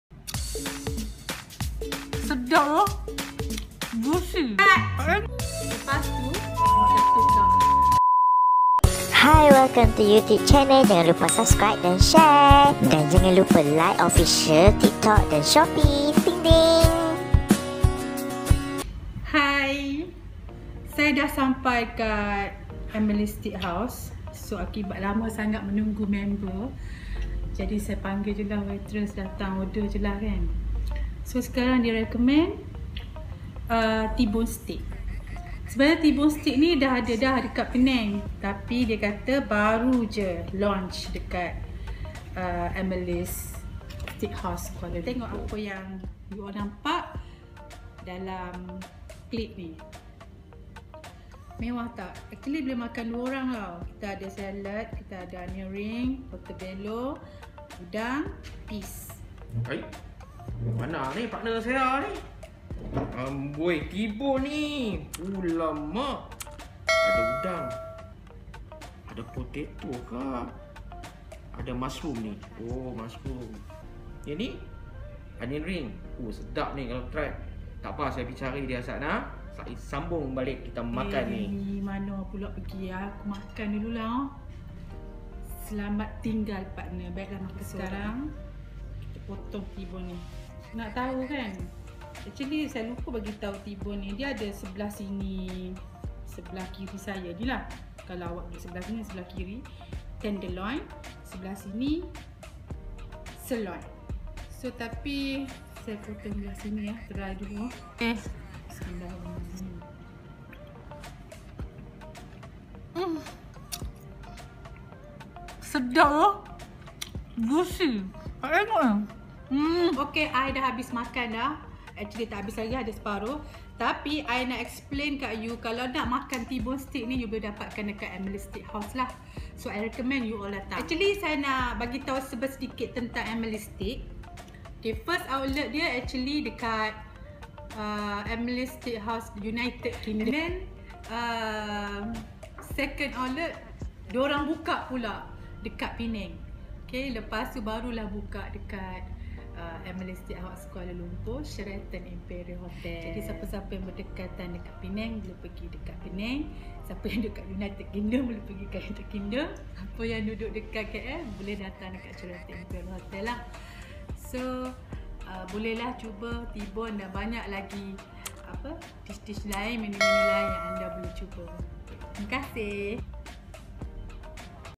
FAMOUS STEAK IN KUALA LUMPUR 2021
𝗪𝗢𝗪! 𝗚𝗼𝗼𝗱 𝗡𝗲𝘄𝘀🎉‼️
 Famous Set at Emily's Peneng is coming  guys!

Calling out all the steak  lovers🔥! 「T-BONE SET」is finally coming to EMILYS KUALA LUMPUR in JUNE 2021😍 Admin is extremely excited and can’t wait to try them out! These are the must-try SHARING menu that you  shouldn’t miss  exclusively bringing you a combination of side dishes  with goodness 🤤!

【𝗠𝗨𝗦𝗧-𝗧𝗥𝗬】
1⃣ T-BONE STEAK

💯 Fresh MEAT from the farm.  unique recipe from UNITED KINGDOM,  Carefully hand-brushed with specialty sauces to perfection. Every bite and moist perfect.

Open everyday from 11am to 3pm, 5.30pm to 10.30pm

📍(KL) Ground Floor, Sheraton Imperial Hotel, Jalan Sultan Ismail, 50250, KL

Open everyday from 11am to 3pm, 5.30pm to 11pm